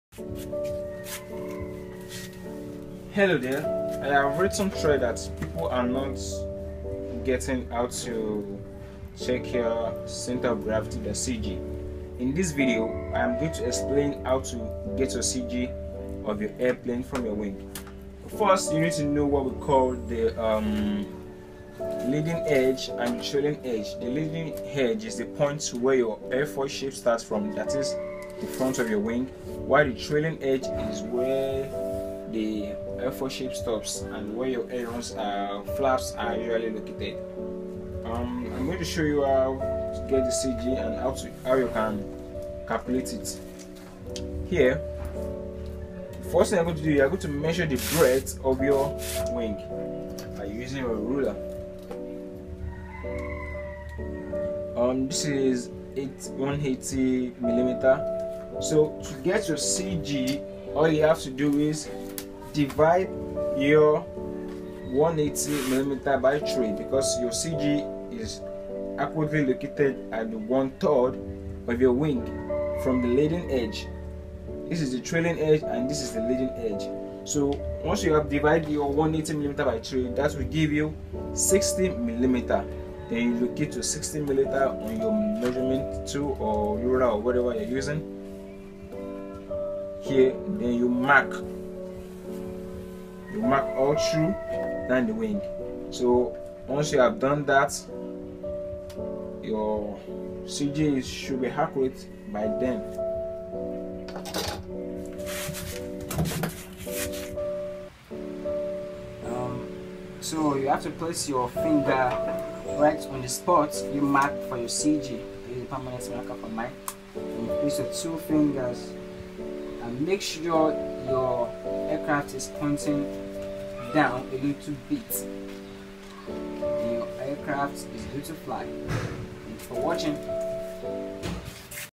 How to find your CG on airplanes
This video explains how you can locate your center of gravity on your airplane. Preferably straight wings as they have the basic way to estimate their CG location.. this method doesn’t apply to swept wing, tapered wing or canards.